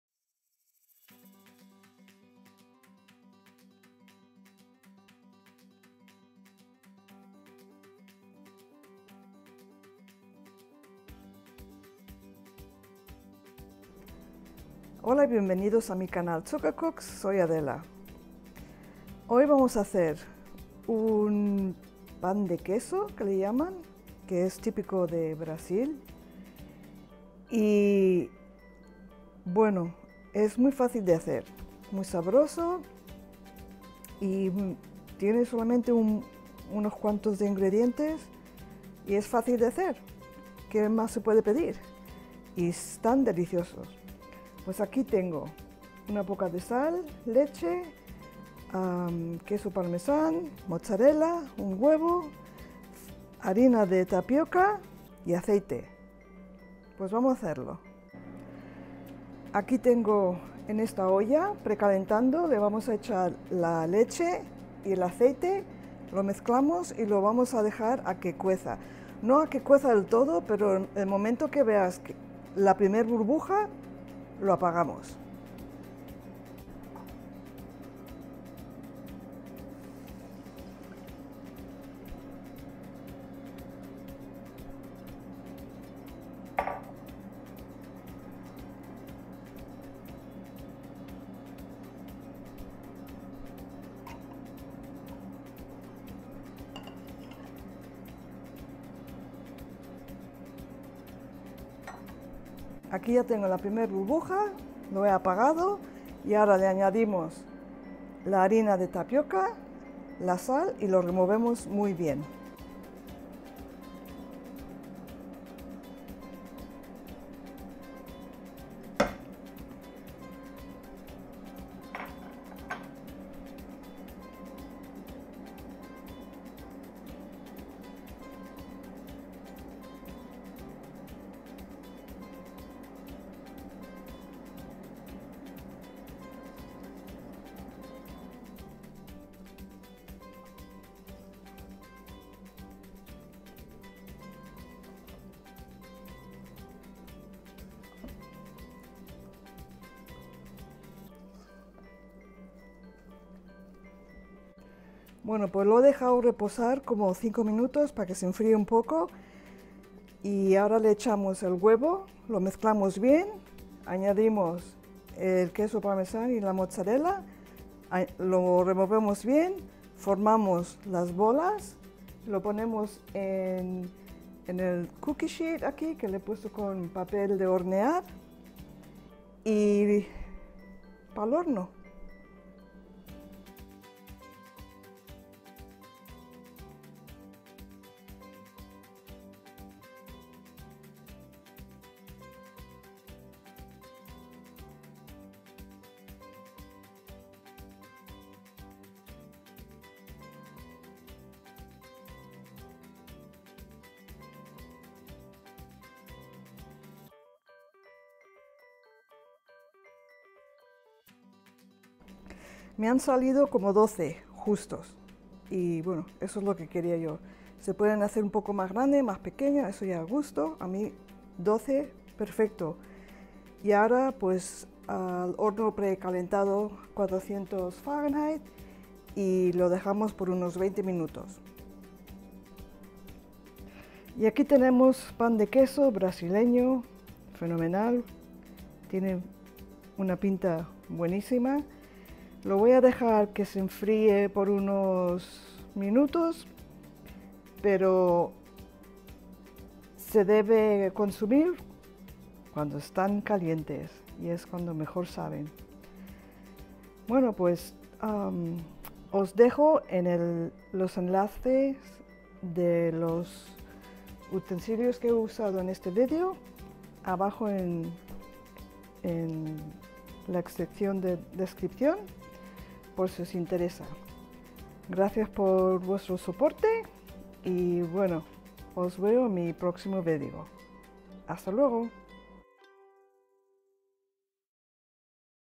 Pan de Queso Brasileño
Receta simple de Pan de Queso Brasileño que te va a encantar. Es facil de hacer y siempre sale estupendo. Pruebalo!
Ingredientes:
3/4 taza de leche entera / 113 G
1/3 taza de aceite de oliva o vegetal / 72 g
1 1/3 tazas de harina tapioca / 170 g
1 huevo
1/2 cucharilla de sal / 7 g
1/3 taza de queso Parmesan / 40 g
2/3 taza de queso mozzarella / 80 g
Hornear a 400F (205C) for 20 minutos o hasta que esten hechos y dorados.

Recomendaciones:
Aqui os dejo enlaces a Amazon de los utensilios que uso en este video, por si os interesa.

Ninja sauce pan Amazon
cookie sheet pan Amazon
Di Oro Silicone Spatula Amazon